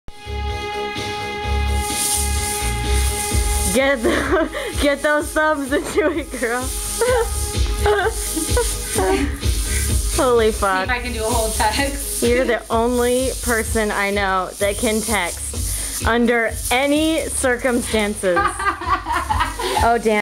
Sarah Jane... Hula expert slash Turbo Philanges
Sarah Jane is like the texting ninja.  She can text, no matter what the circumstances.  Here she is in all of her texting glory.